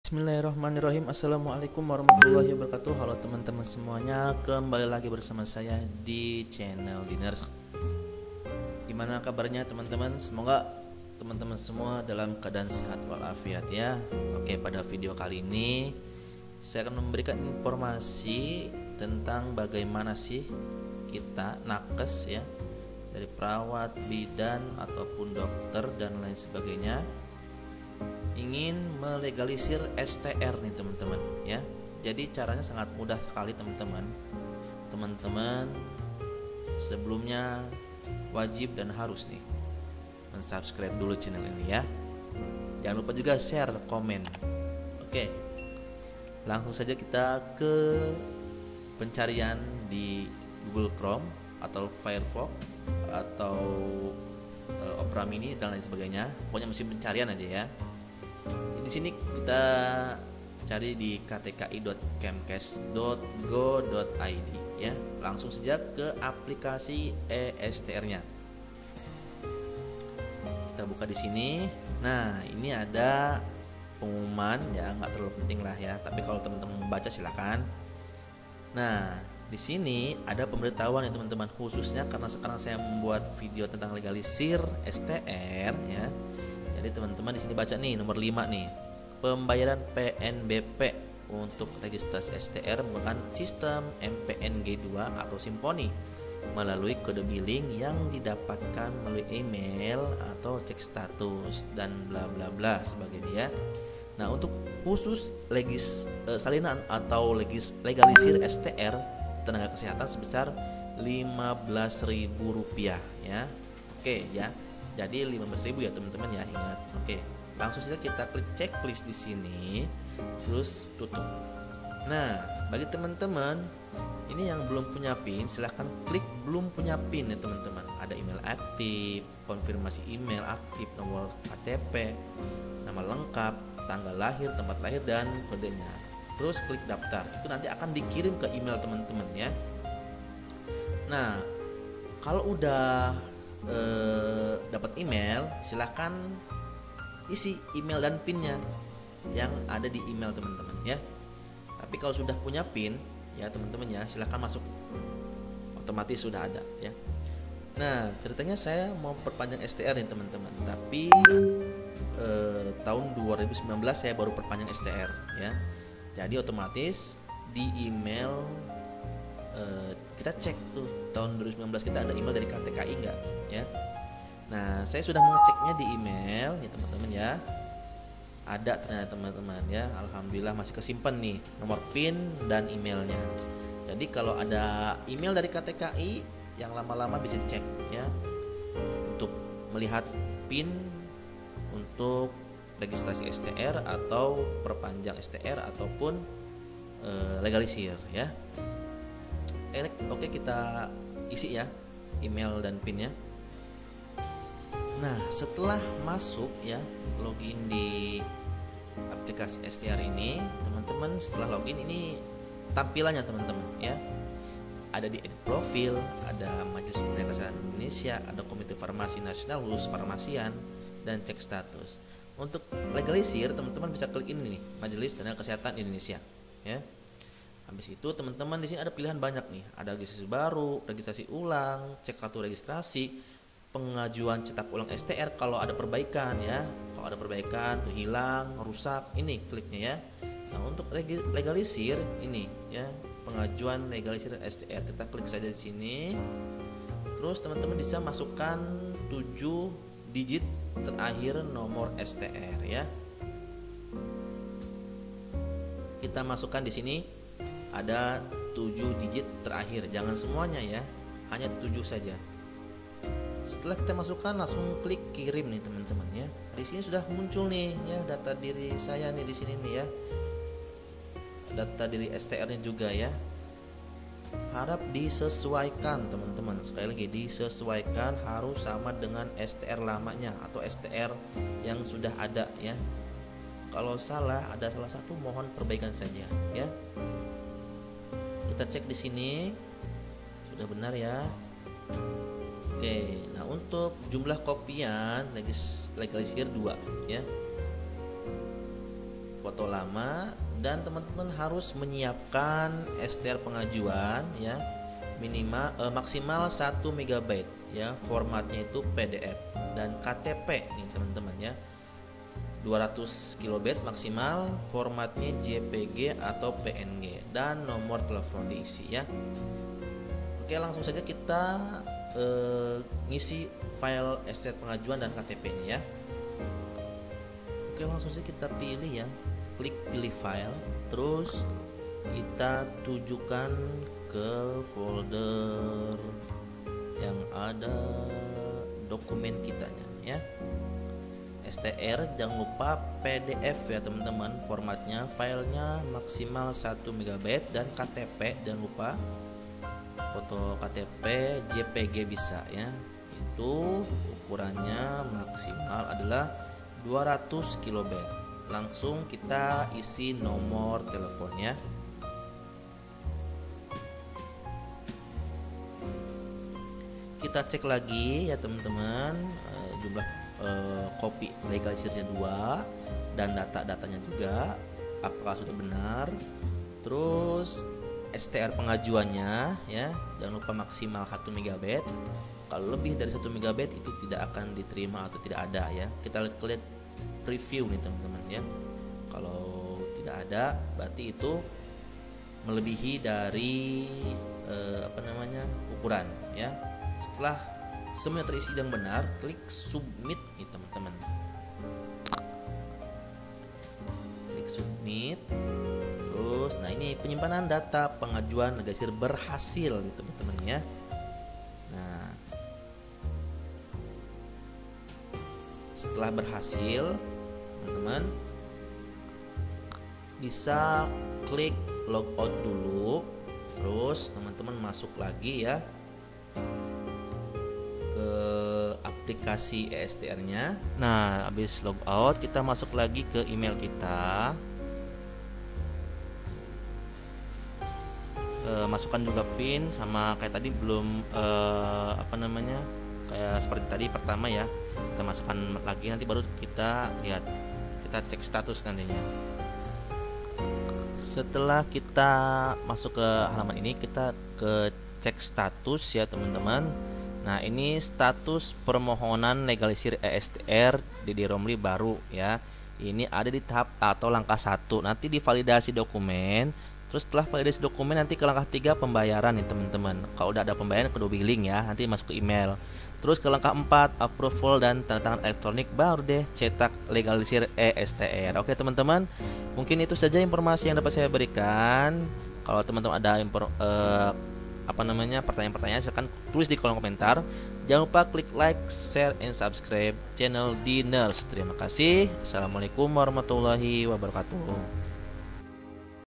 Legalisir E-STR Terbaru
Pada video kali ini saya akan memberikan informasi tentang Cara Legalisir E-STR 2022

Terimakasih..

serba serbi tentang ilmu keperawatan, tindakan keperawatan dan tenaga medis dan paramedis lainnya, serat info-info terkini mengenai dunia kesehatan